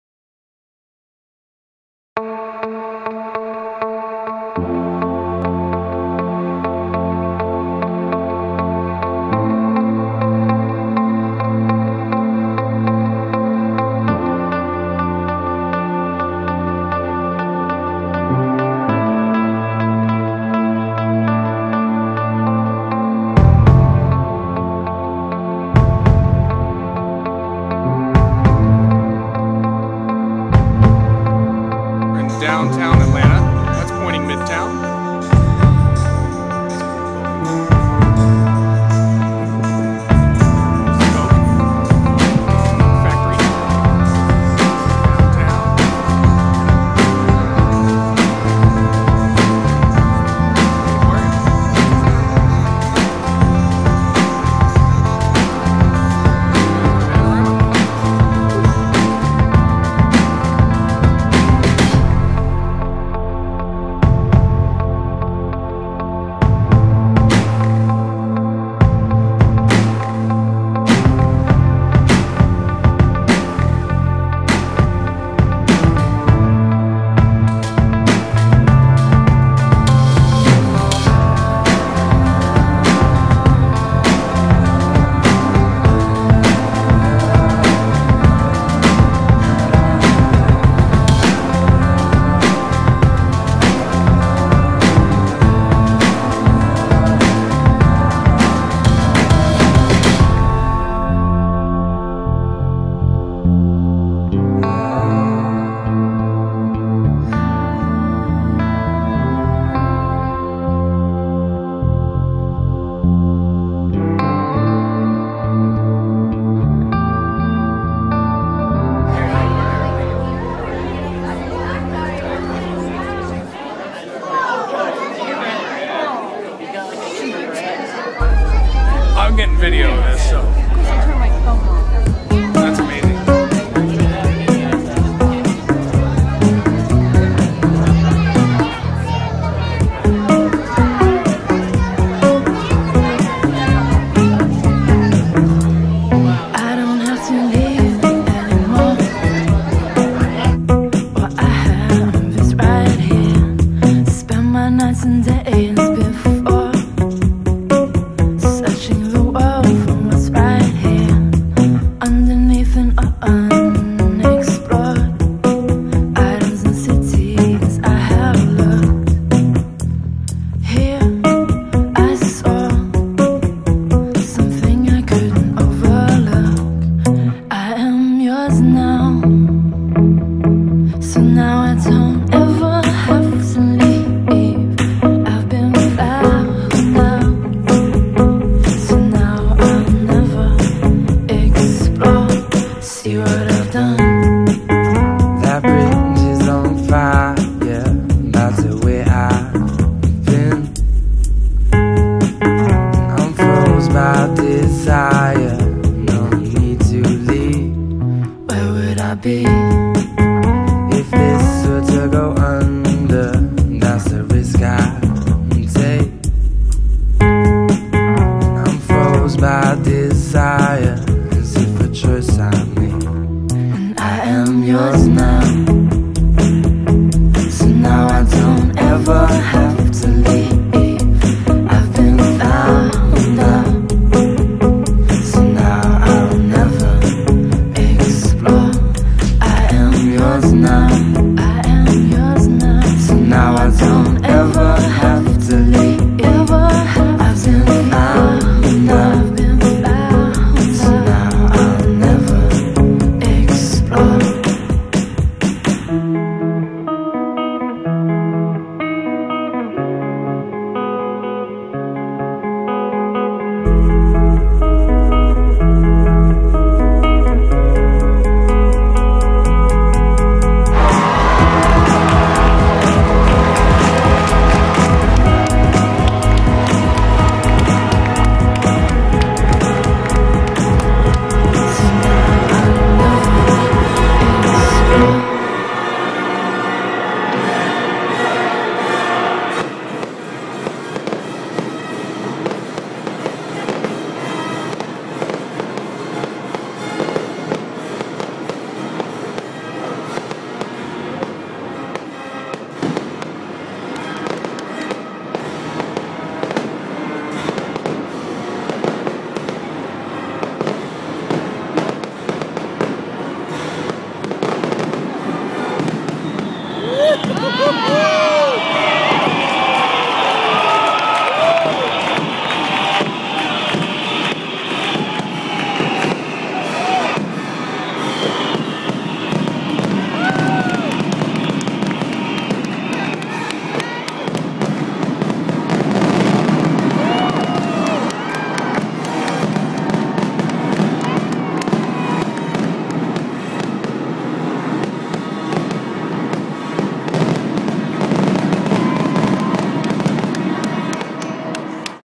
Graham Fam Trip to ATL
The Graham family take a trip to downtown Atlanta for a great time at the aquarium and Braves game for the 4th of July.  Check out the fireworks at the end.